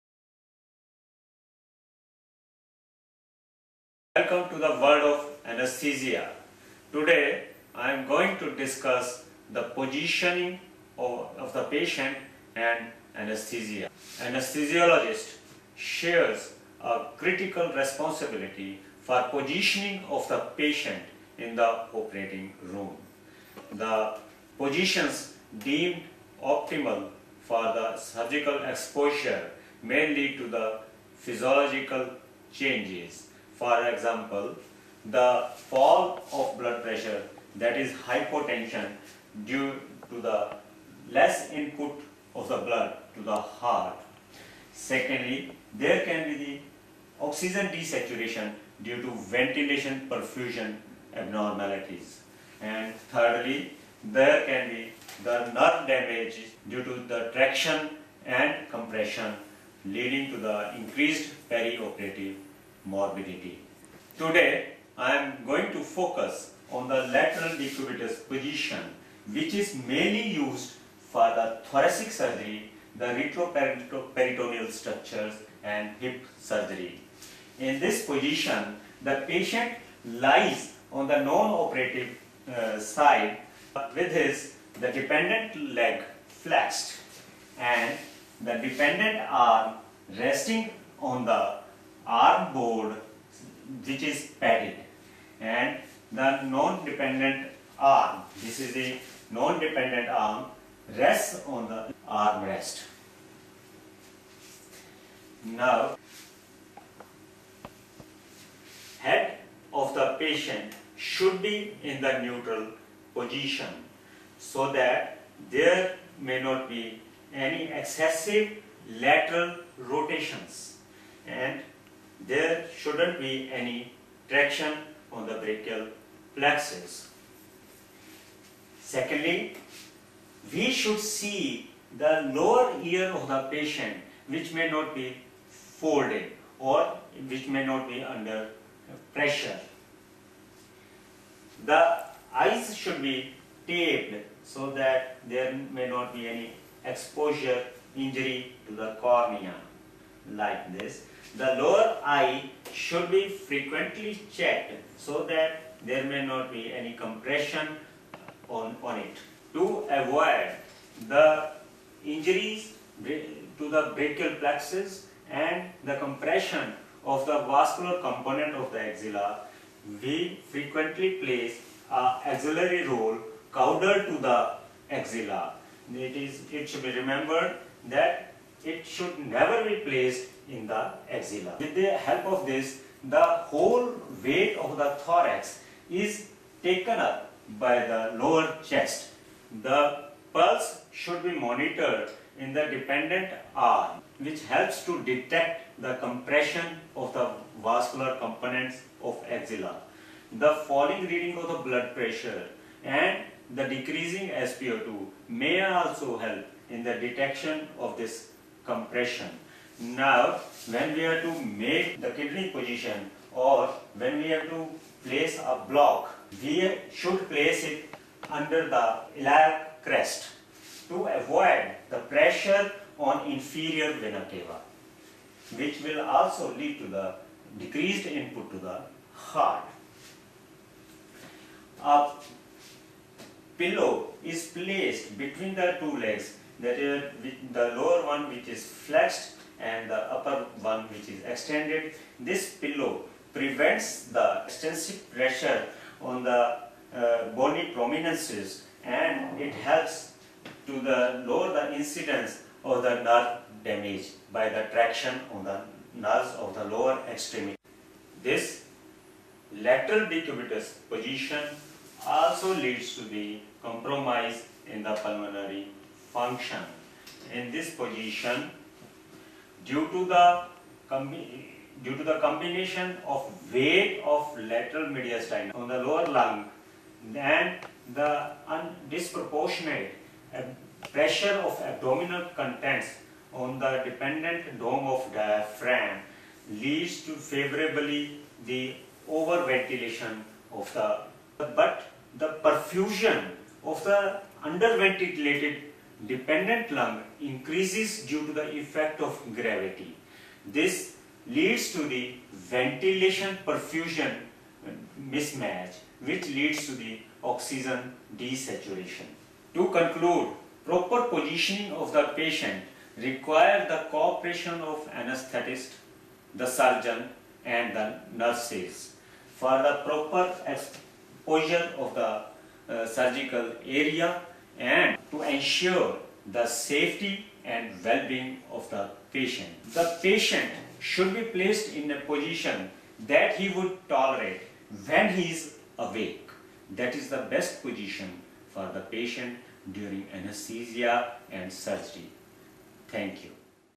Anesthesia world- Positioning of Patient And Anesthesia
Optimal positioning during Anesthesia is required to ensure the well being and safety of patient and also for the optimal exposure for surgery